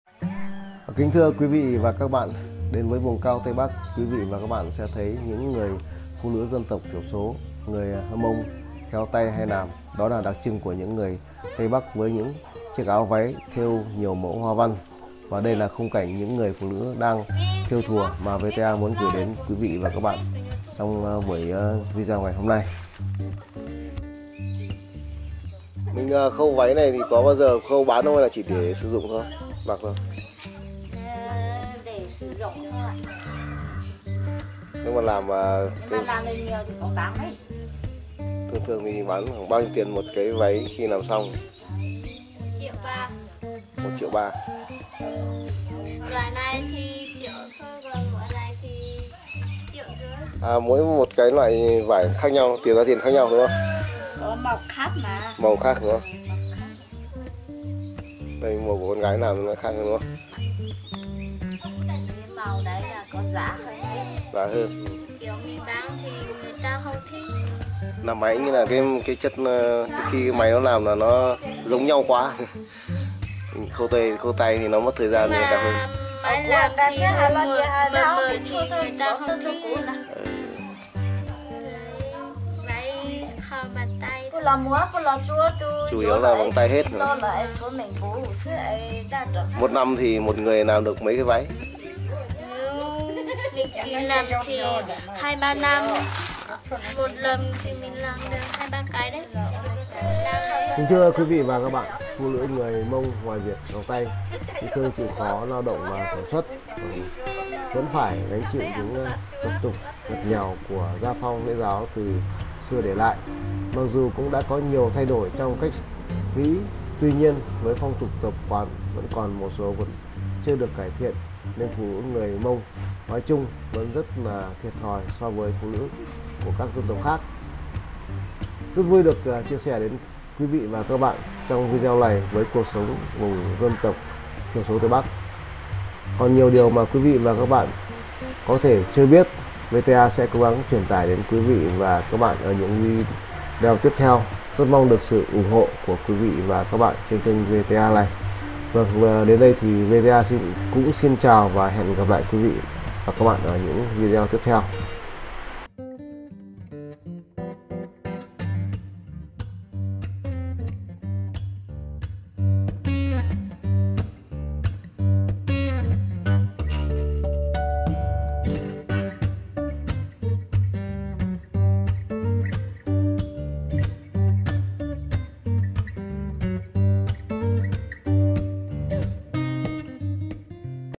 KHÁM PHÁ VIỆT NAM, CUỘC SỐNG VÀ BÀN TAY KHÉO LÉO CỦA CÔ GÁI DÂN TỘC , TÂY BẮC
TÌM HIỂU CUỘC SỐNG TÂY BẮC /PHỤ NỮ  NGƯỜI MÔNG VÙNG CAO THÊU HOA VĂN  ÁO, VÁY
những bàn tay khéo léo của người phụ nữ tây bắc , trong công đoạn làm đẹp cho trang phục